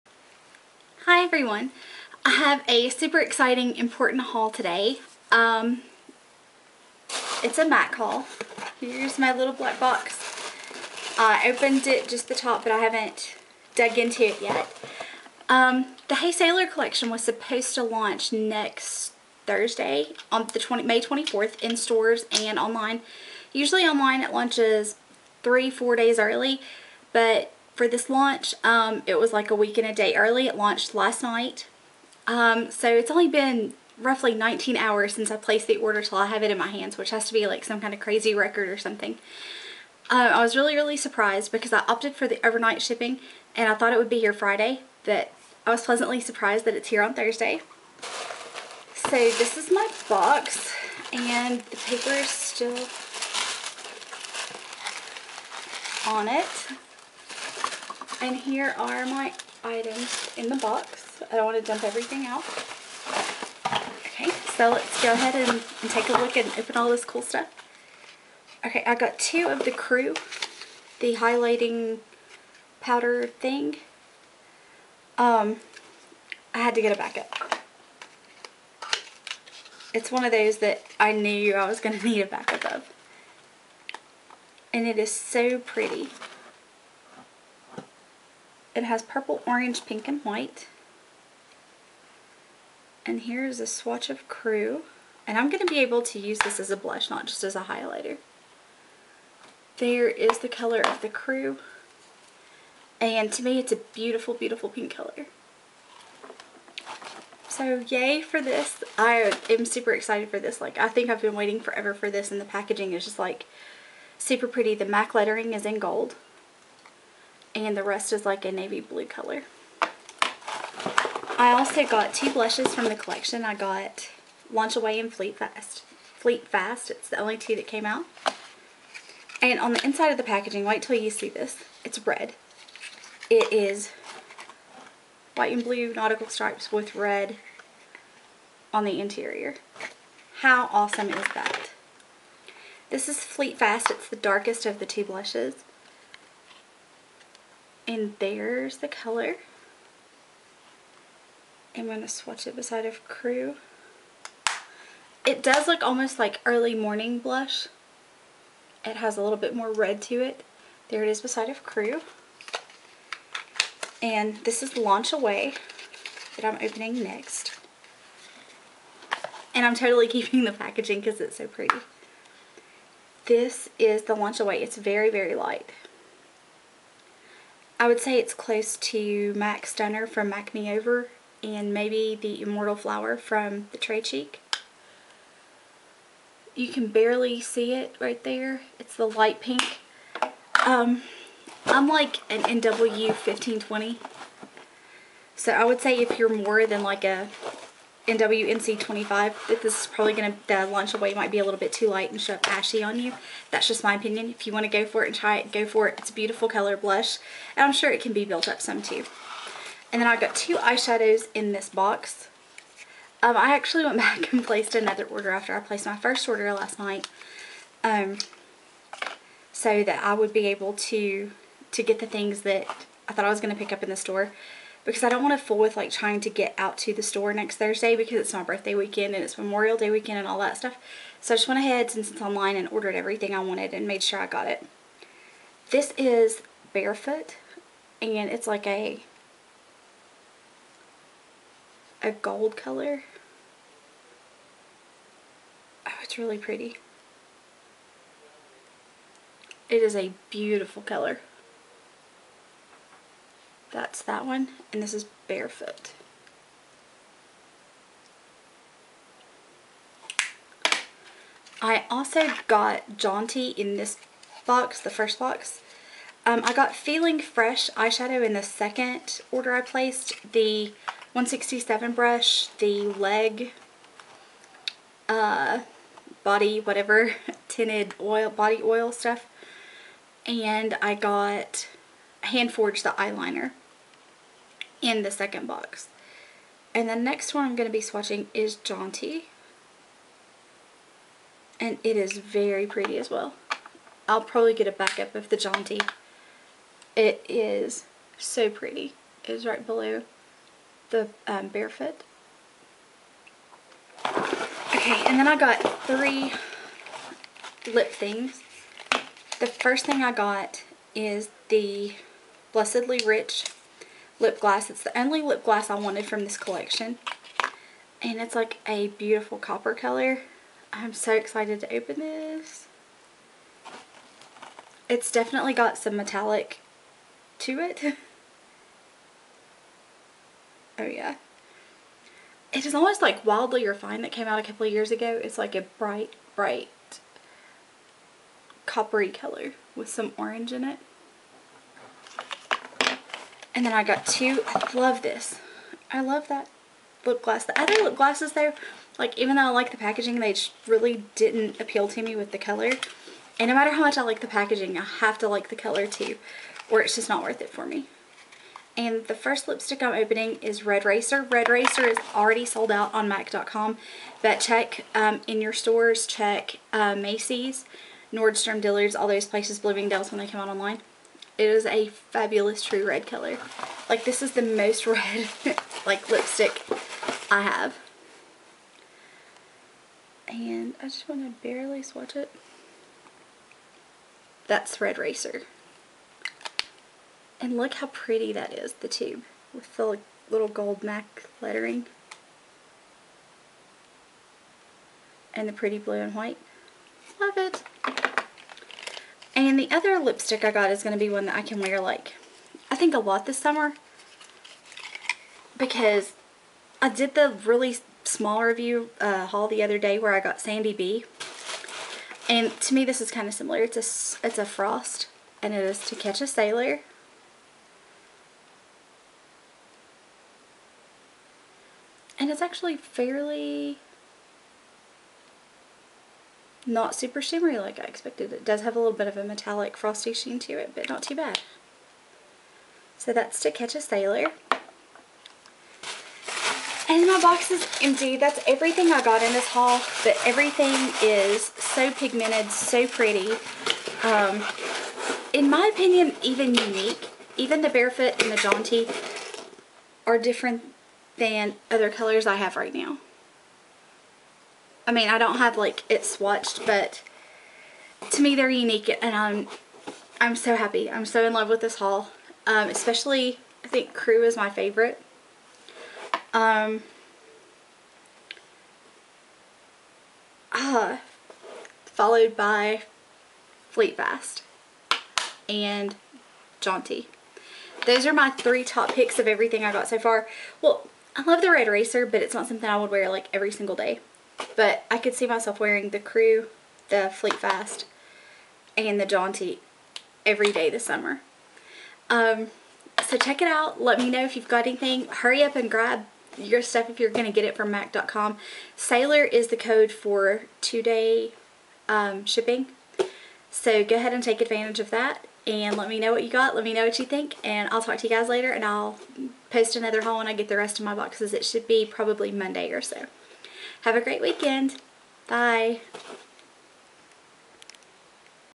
MAC Hey Sailor! Haul!!!
My haul, mini review, and swatches from the new Hey Sailor! collection.  I love this collection! Everything I got is superb in color and quality!!